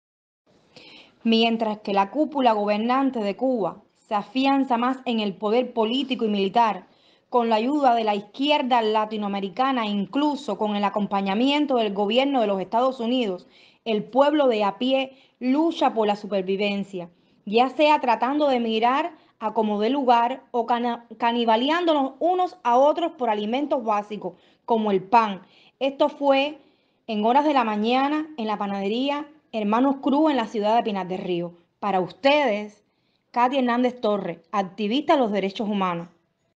Denuncia la falta de alimentos en Cuba
Pinar del Río,Cuba- 17 de enero del 2023 - Denuncia la activista de derechos humanos, Katia Hernandez la falta de alimentos en Cuba , especificamente la falta de pan en Pinar del Río.